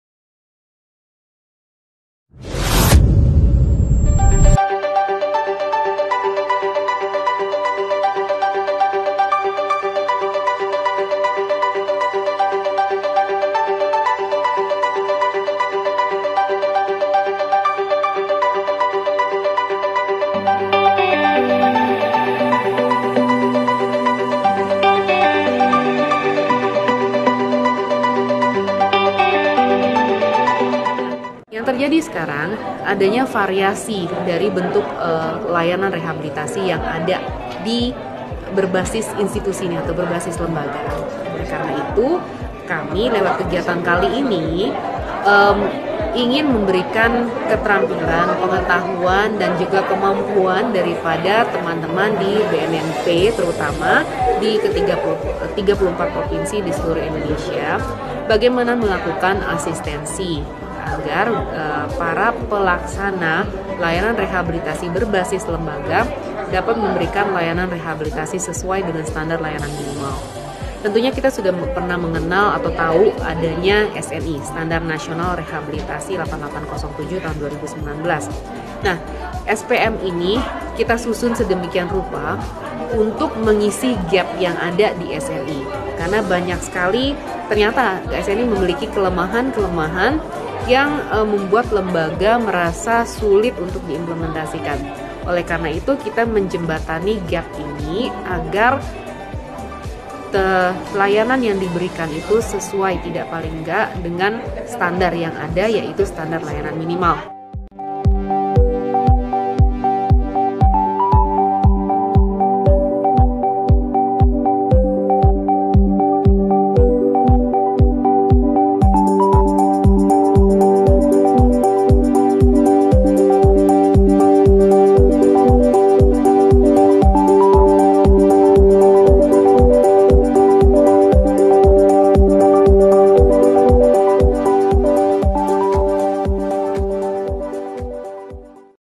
JAMINAN MUTU DAN AKUNTABILITAS LAYANAN REHABILITASI MELALUI SPM
Direktorat PLRKM Deputi Bidang Rehabilitasi BNN selenggarakan giat bintek dan Penilaian Layanan Rehabilitasi sesuai Standar Pelayanan Minimal (SPM) bagi petugas rehabilitasi BNN Provinsi se Indonesia yang dilaksanakan di IPB International Convention Center, Bogor, Jawa Barat,  (15/6).
Dalam penanganan lahgun narkoba, rehabilitasi miliki peranan yang sangat penting. Proses pemulihan pecandu atau lahgun narkoba melalui rehabilitasi, secara siginifikan dianggap dapat tekan laju angka prevalensi lahgun narkoba.
dr. Amrita Devi, SpKJ, M.Si, Direktur PLRKM sekaligus Plt.Deputi Bidang Rehabilitasi BNN katakan bahwa saat ini terdapat variasi dari bentuk layanan rehabilitasi khususnya dari lembaga rehabilitasi berbasis institusi.
“Oleh karena itu, kami ingin beri keterampilan, pengetahuan, dan kemampuan bagi pelaksana layanan rehabilitasi di 34 BNN Provinsi sehingga para pelaksana rehabilitasi berbasis institusi dapat memberi layanan rehabilitasi sesuai dengan standar pelayanan minimal", ujar dr.Amrita Devi.
Standar Pelayanan Minimal (SPM) layanan rehabilitasi adalah ketentuan mengenai jenis dan mutu layanan dasar rehabilitasi yang wajib dipenuhi oleh lembaga rehabilitasi sehingga akan jamin mutu dan akuntabilitas layanan yang diberikan sebagai bentuk pertanggungjawaban lembaga rehabilitasi kepada masyarakat.
SPM ini disusun sebagai tahap awal bagi lembaga rehabilitasi untuk persiapkan layanan menuju Standar Nasional Indonesia (SNI) Nomor 8807:2019 tentang Penyelenggaraan Layanan Rehabilitasi.
Menurut dr.Amrita Devi, SNI merupakan standar yang ideal miliki beberapa kelemahan yang butuhkan waktu panjang dan keterampilan khusus bagi penyelenggara layanan rehabilitasi untuk implementasikannya dalam layanan yang diberikan kepada penyalah guna narkoba.
"Ternyata SNI miliki kelemahan yang membuat lembaga sulit implementasikan, oleh karena itu Deputi Rehabilitasi jembatani hal ini dengan SPM sehingga lembaga rehabilitasi dapat berikan layanan dengan standar minimal",tutup dr.Amrita Devi.
Temukan kami di :
Youtube : BNN Kabupaten Purbalingga